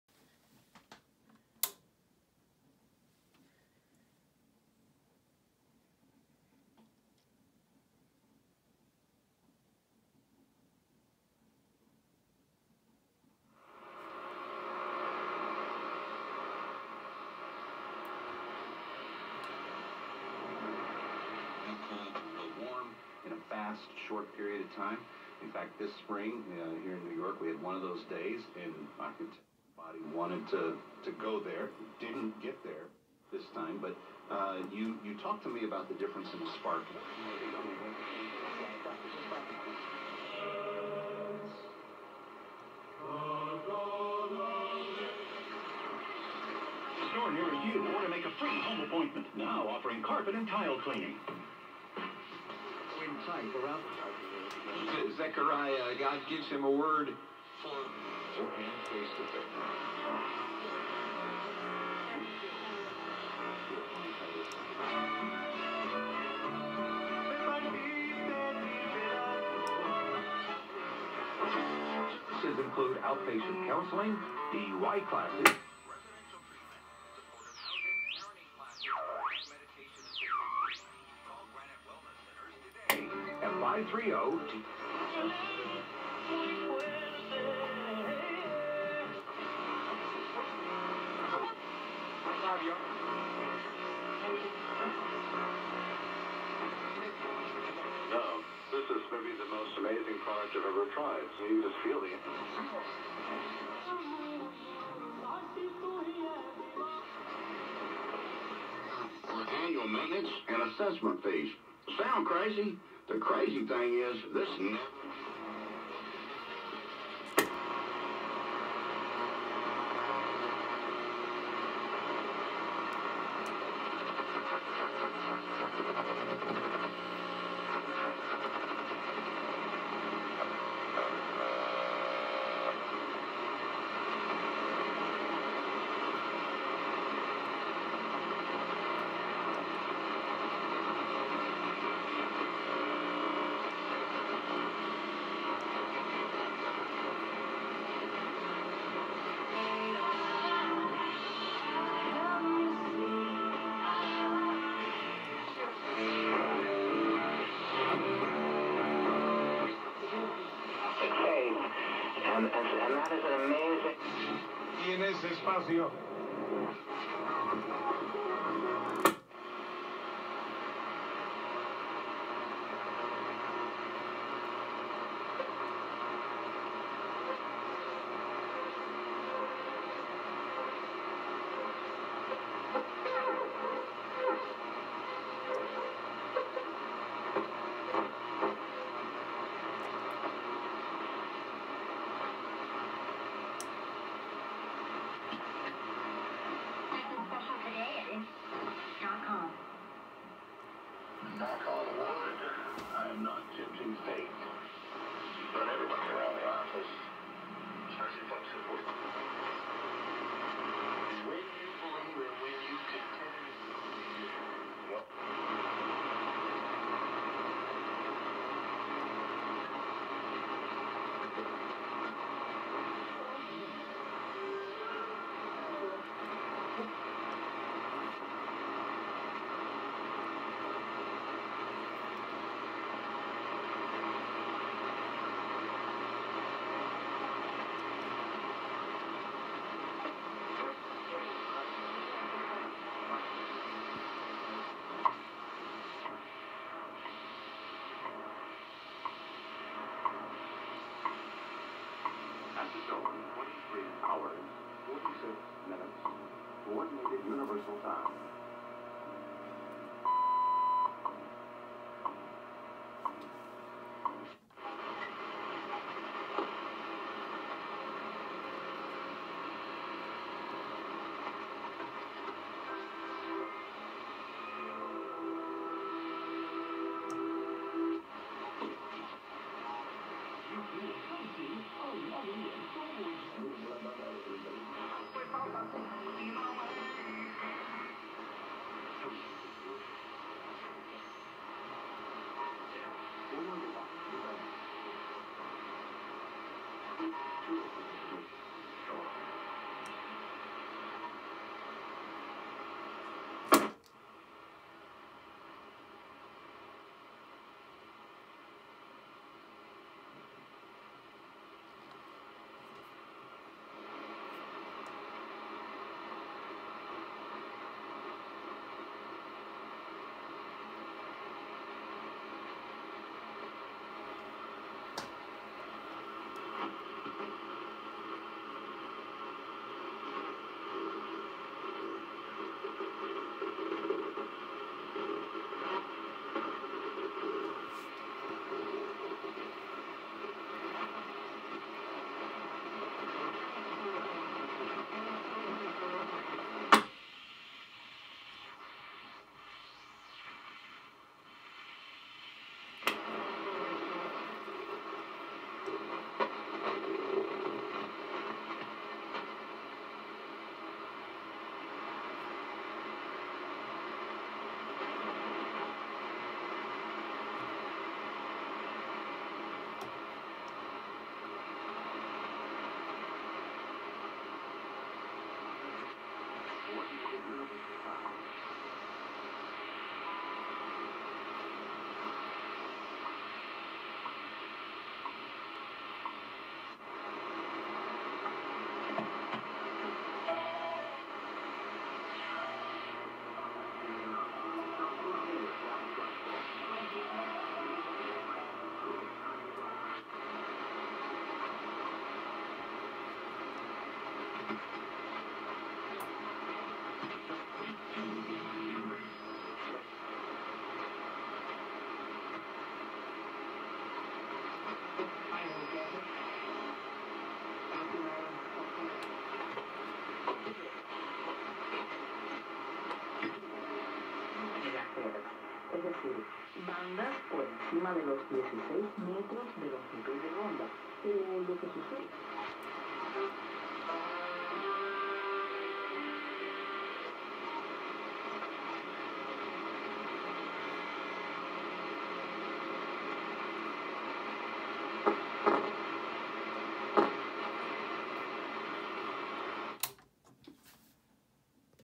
National NC-121 tuning through all four bands
Selling this nice little NC-121. I tune through all four bands, starting at the BCB. The antenna is about 100 feet of wire up in the trees at my QTH in Camino, California. The time was about 3:30 PM, an hour or so before sunset.